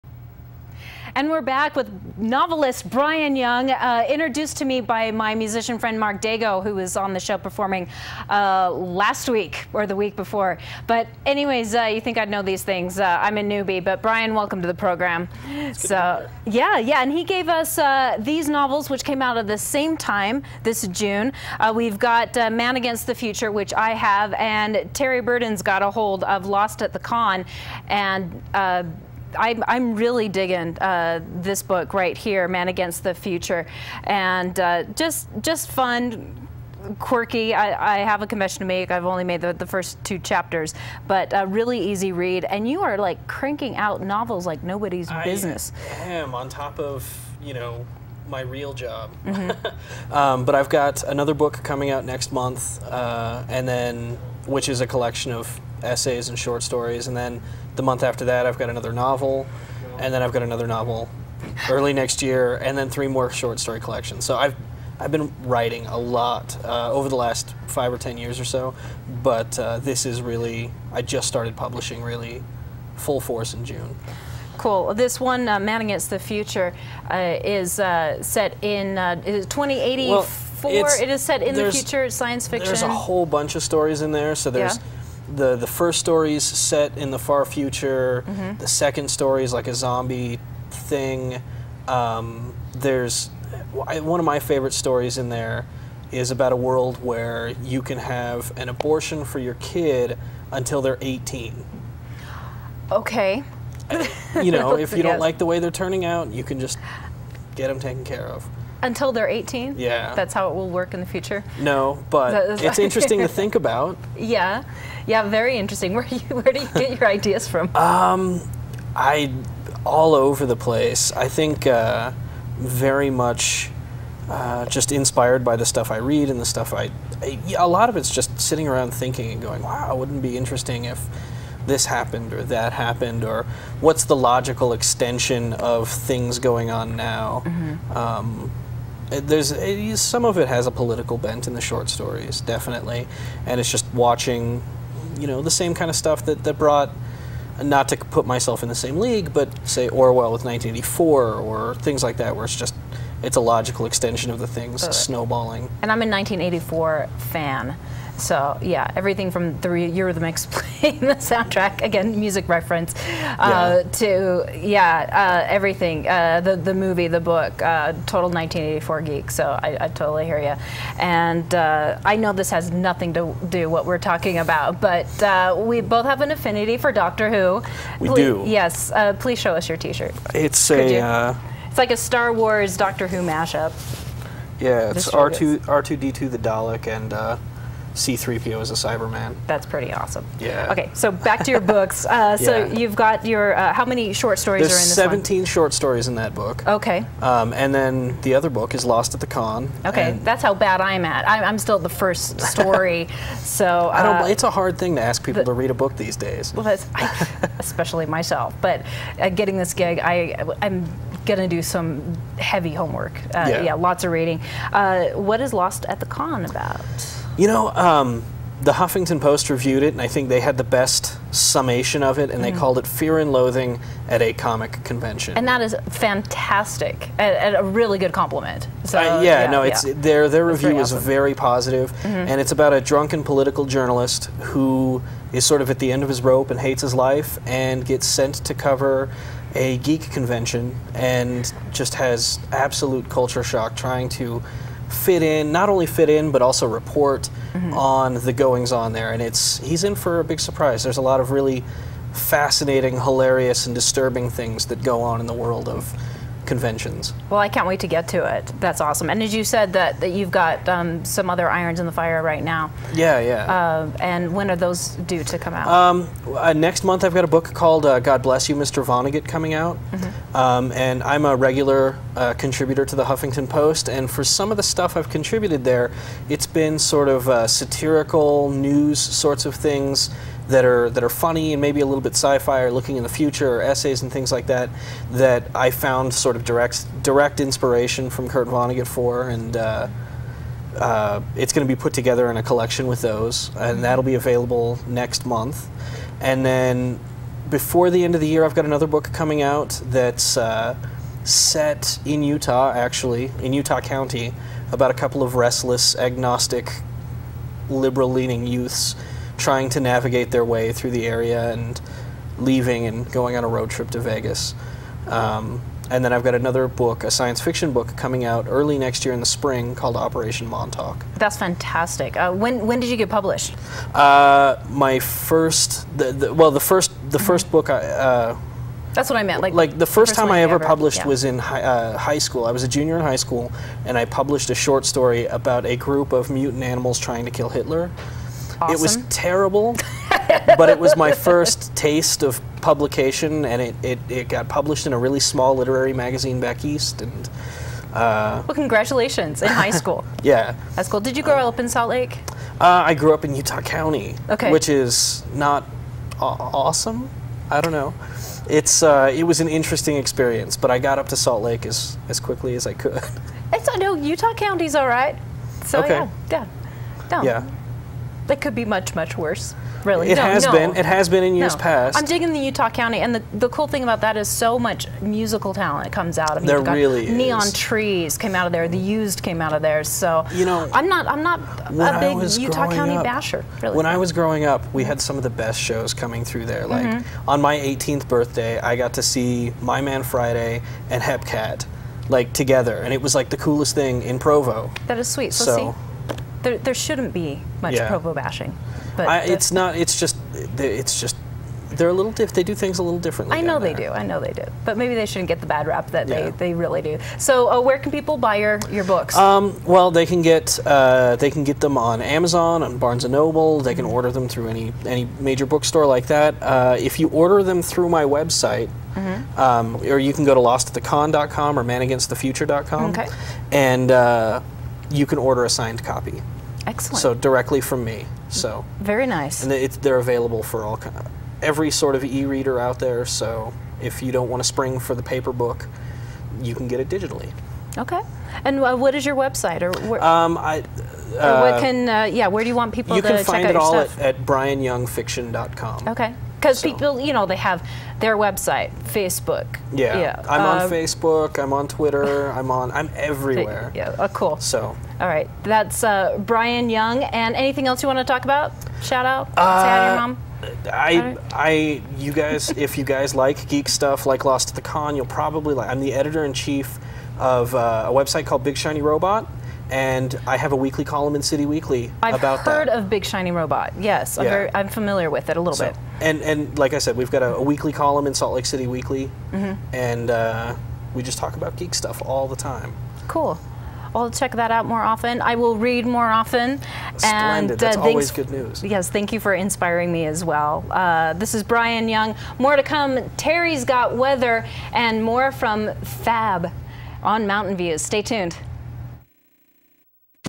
Bryan Young talks about Lost at the Con on Park City TV
Bryan Young appeared on Park City TV's Mountain Views to talk about his writing and his upcoming projects.